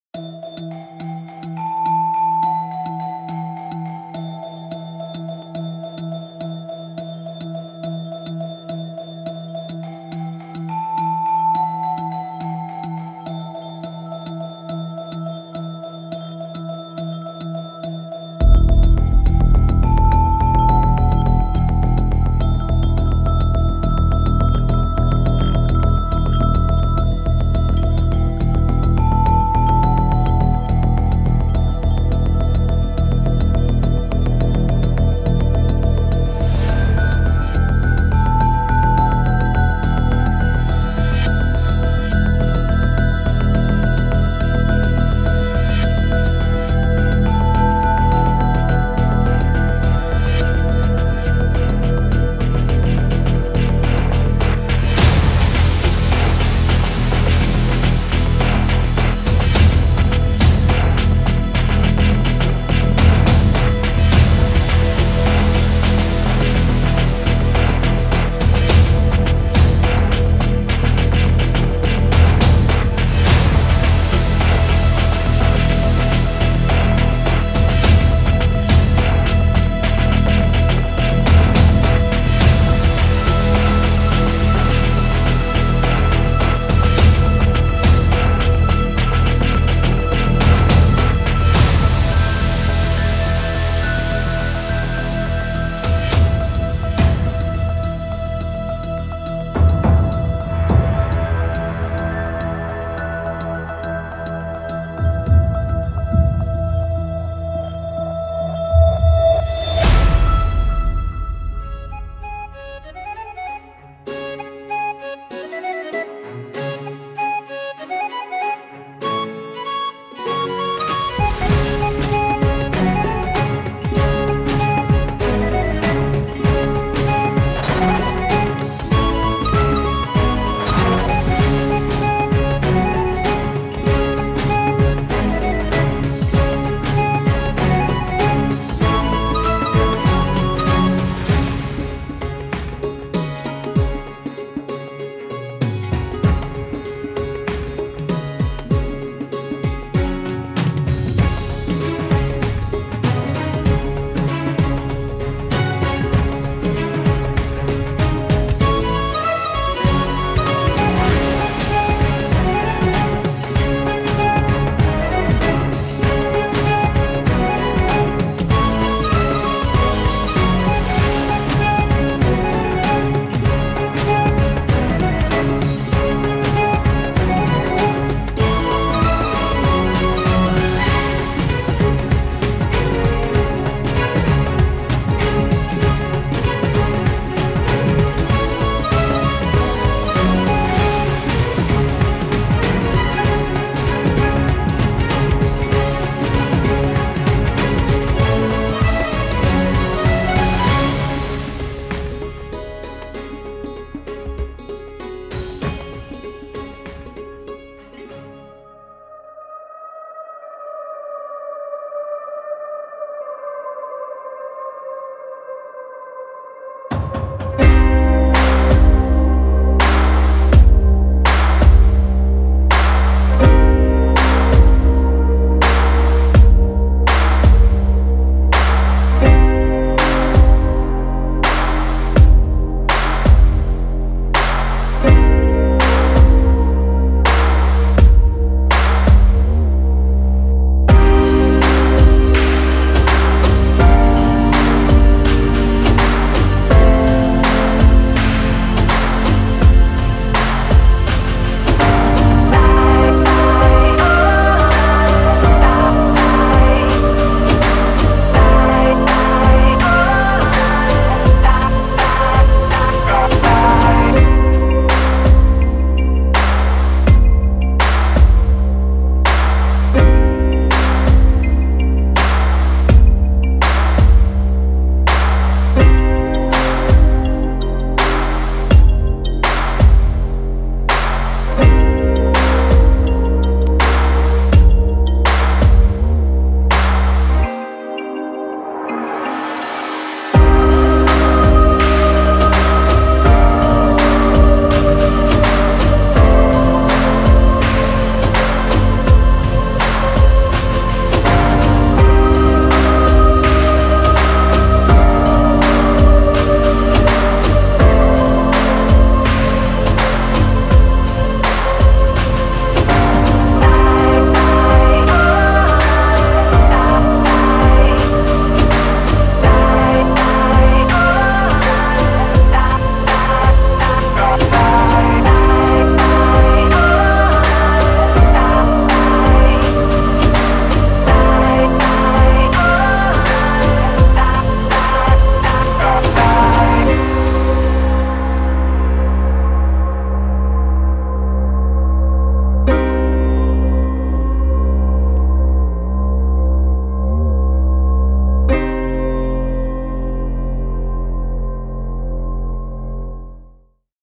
Phoenix Timelapse Art | Photoshop
A timelapse photoshop edit of a phoenix. This is kind of a "re-do" of a previous phoenixphotoshop video I've made that I wasn't really happy with, despite that one being my most viewed video. Hopefully this one hits the mark. Thanks for watchin'!